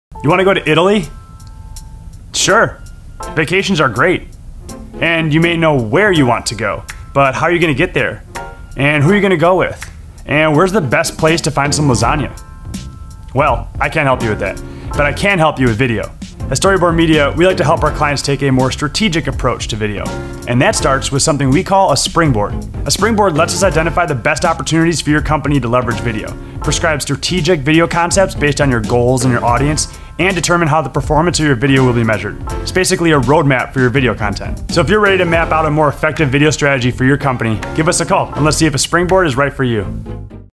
“The Springboard” | A Video Roadmap | Video Strategy Initiatives
Take a more strategic approach to video. The Springboard is our proprietary video strategy process that gives you everything you need to practice effective video for your business.
A Springboard helps us identify the best opportunities for your company to leverage video, prescribe strategic video concepts based on your audience and goals, and determine how the performance of your video will be measured. It’s essentially a roadmap for your video content.